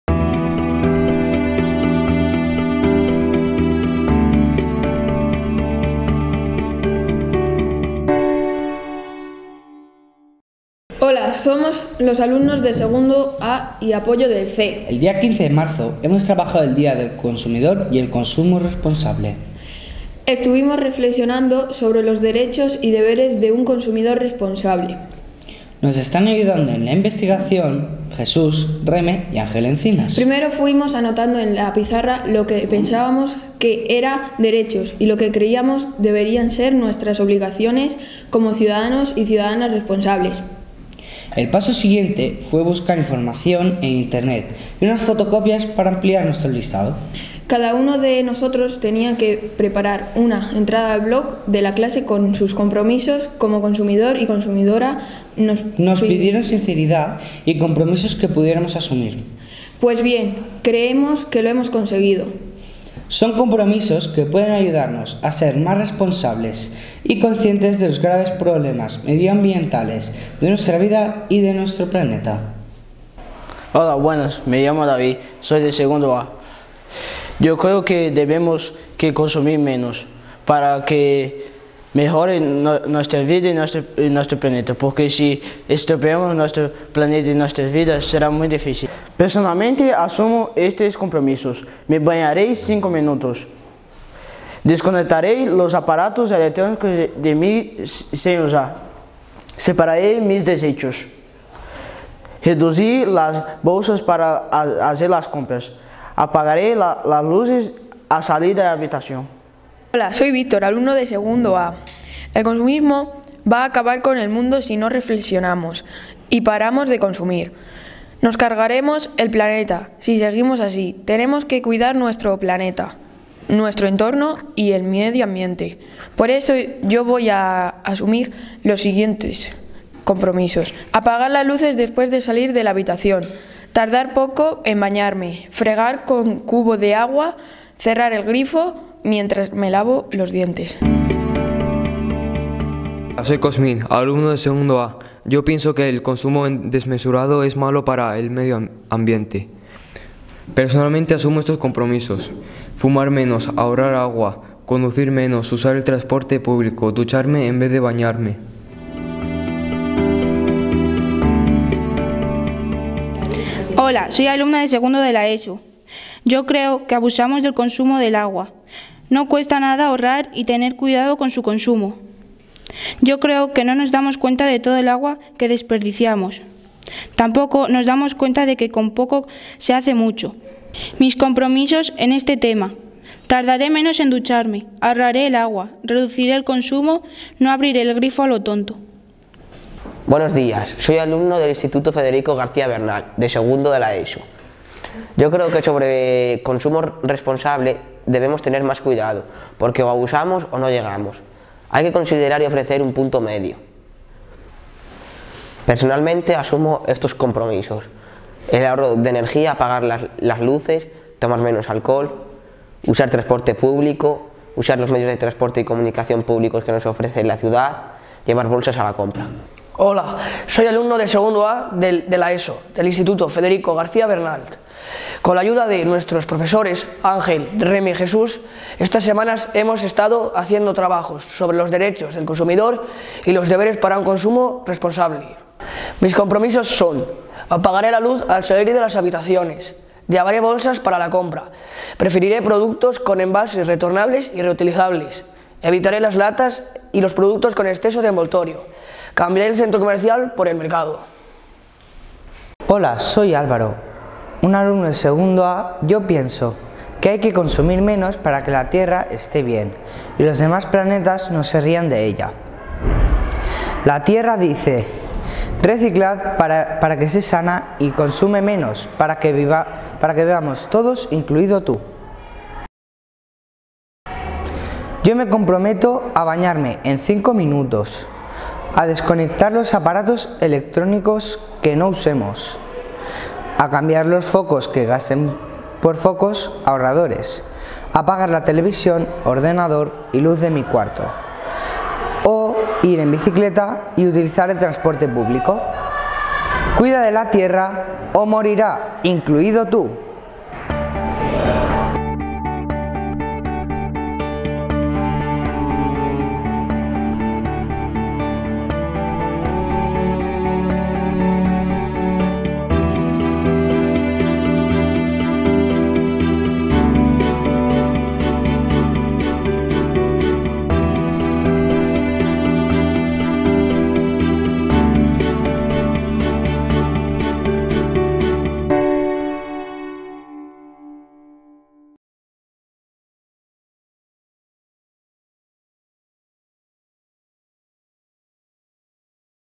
15 de marzo, Día de Consumidor. Nuestros compromisos personales
Este vídeo es el resultado de un trabajo en torno a los derechos del consumidor y los compromisos de un consumidor responsable.  Se puede ver en esta dirección